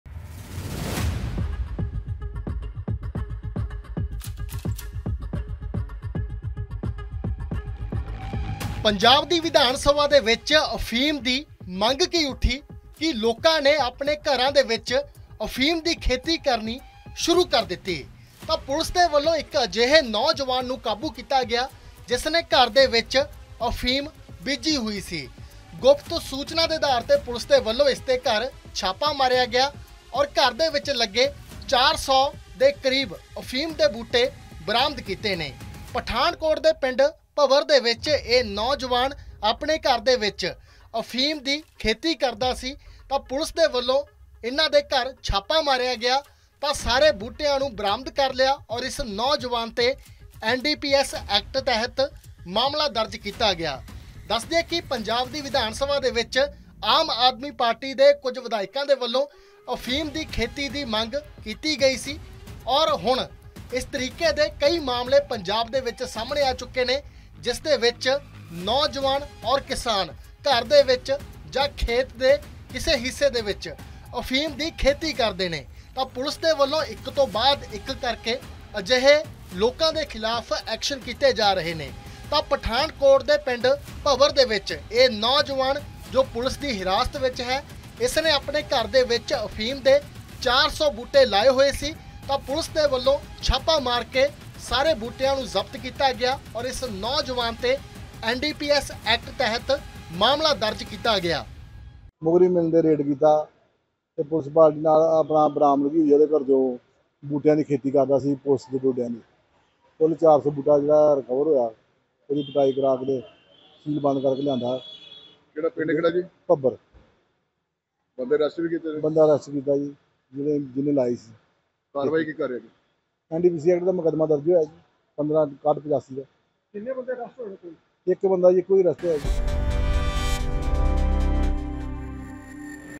Pathankot News | ਚੁੱਪ-ਚਪੀਤੇ ਨੌਜਵਾਨ ਨੇ ਘਰੇ ਬੀਜੇ ਅਫ਼ੀਮ ਦੇ 400 ਬੂਟੇ, ਪੁਲਿਸ ਨੇ ਮਾਰਿਆ ਛਾਪਾ
ਸੂਬਾ ਸਰਕਾਰ ਵਲੋਂ ਨਸ਼ਿਆਂ ਨੂੰ ਖਤਮ ਕਰਨ ਦੇ ਲਈ ਅਣਥੱਕ ਪਰਿਆਸ ਕੀਤੇ ਜਾ ਰਹੇ ਨੇ ਤਾਂ ਜੋ ਪੰਜਾਬ ਦੀ ਜਵਾਨੀ ਨੂੰ ਨਸ਼ਿਆਂ ਤੋਂ ਬਚਾਇਆ ਜਾ ਸਕੇ ਪਰ ਕੁਝ ਨਸ਼ੇ ਦੇ ਸੌਦਾਗਰ ਅੱਜ ਵੀ ਪੰਜਾਬ ਦੀ ਜਵਾਨੀ ਨੂੰ ਨਸ਼ਿਆਂ ਵੱਲ ਧਕਣ ਤੋਂ ਪਿੱਛੇ ਨਹੀਂ ਹਟ ਰਹੇ।